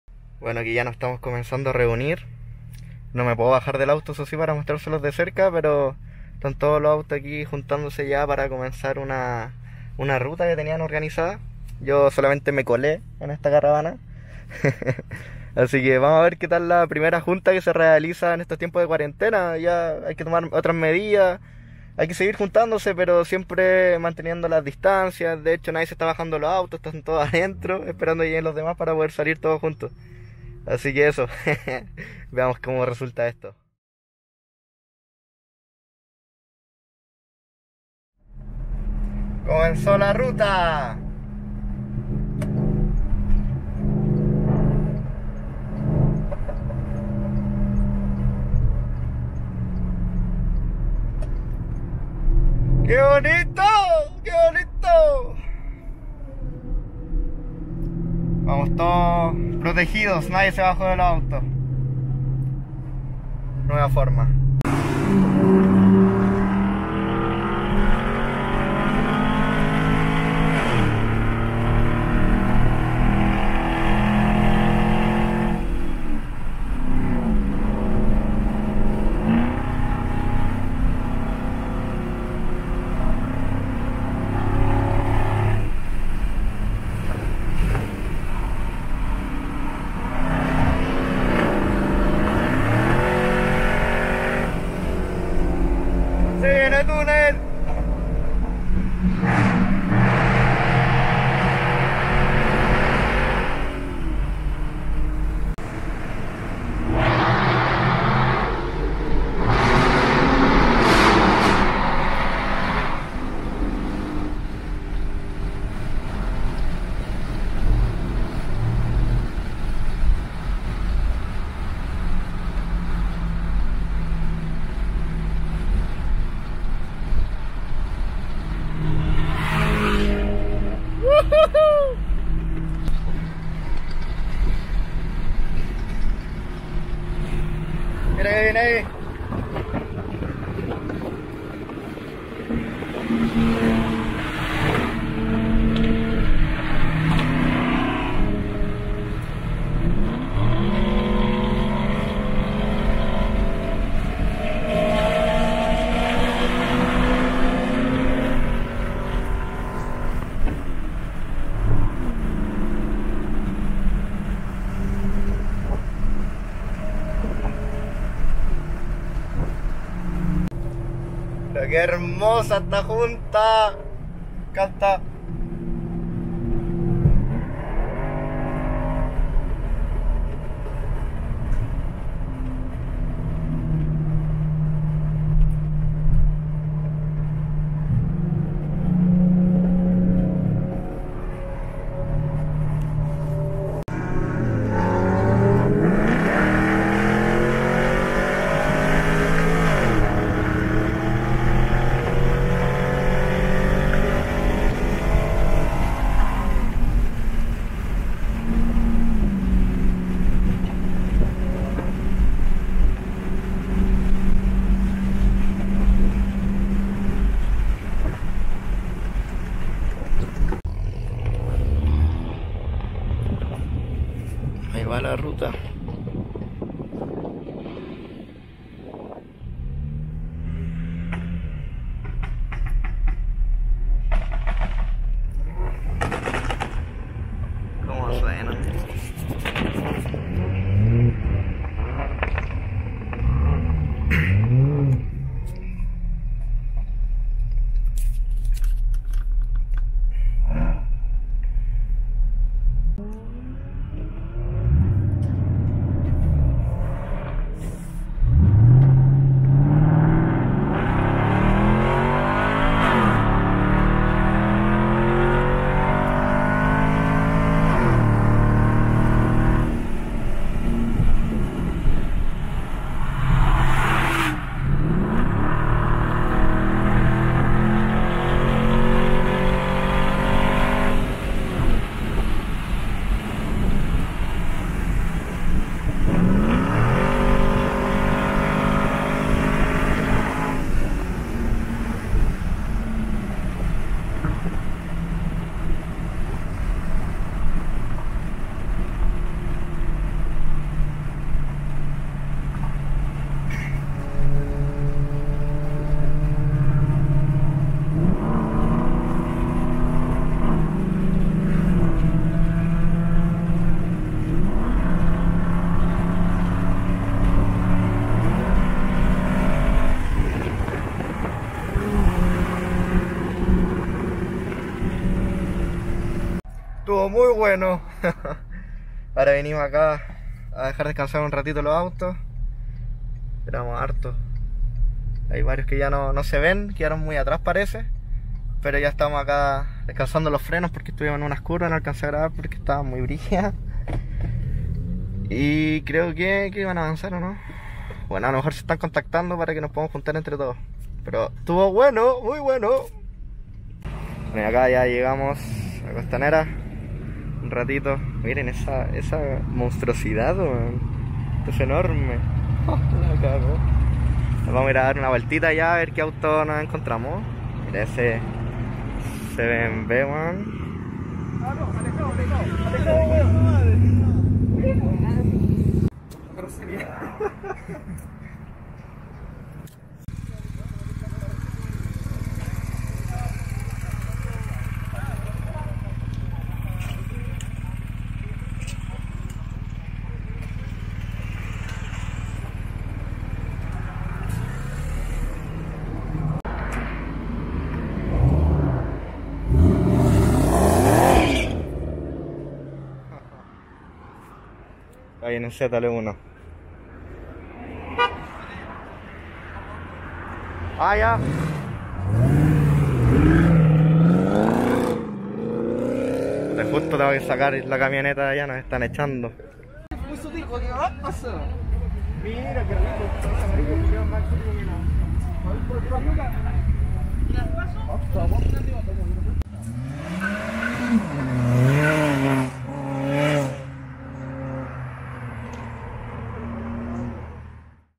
CARAVANA DE DEPORTIVOS en CUARENTENA
debido a la contingencia que hemos estado viviendo, una organización decidio armar una caravana donde la idea era solo distraernos un rato, sin contacto social para evitar problemas mayores.
PD: respetamos todos los limites de velocidad correspondientes a la ley